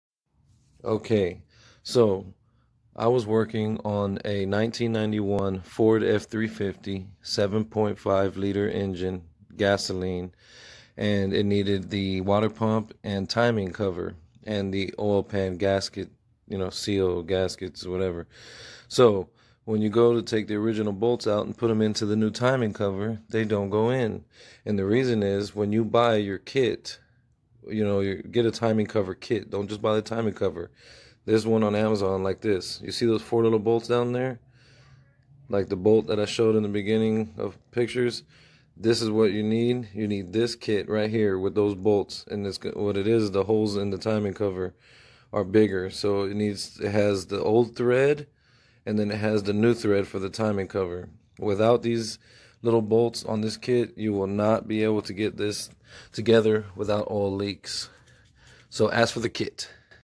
7.5l 1991 Ford F350 Replace Timing Cover Help FIX
To install revised bolts properly oil pan needs to be dropped

Dorman 635-101 Engine Timing Cover